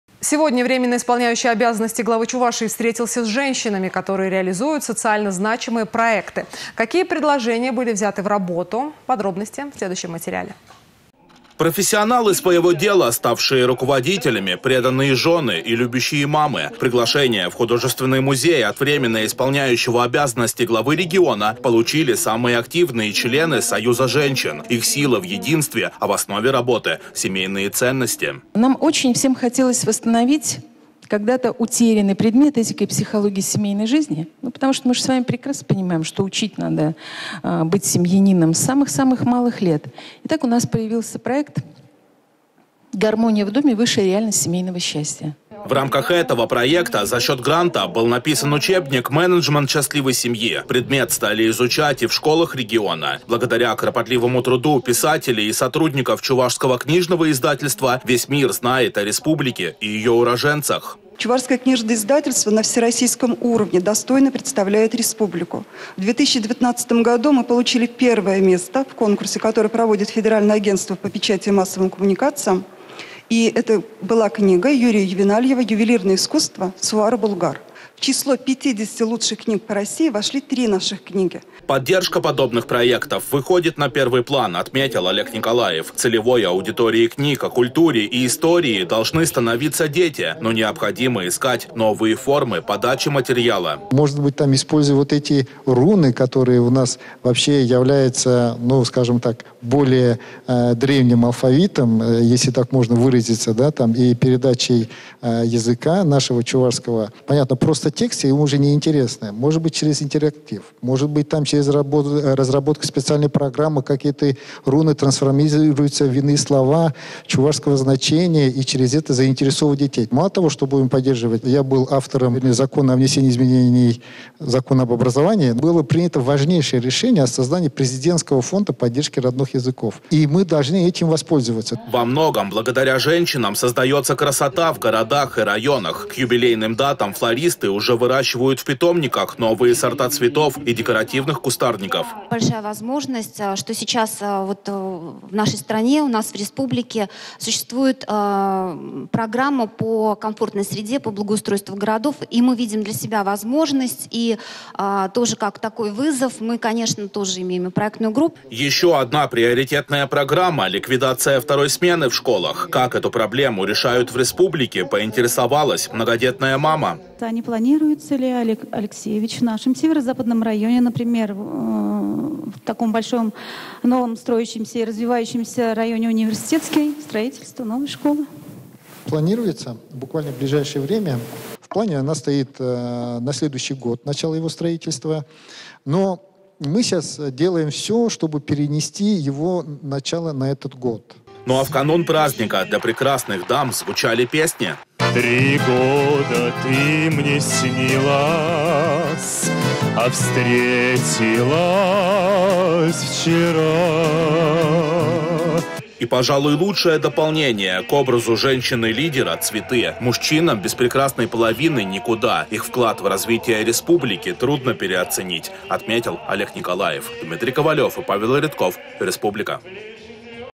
Врио Главы Чувашии Олег Николаев встретился с Женсоветом Чувашии
Временно исполняющий обязанности Главы Чувашии встретился с женщинами, которые реализуют социально значимые проекты. Какие предложения были взяты в работу?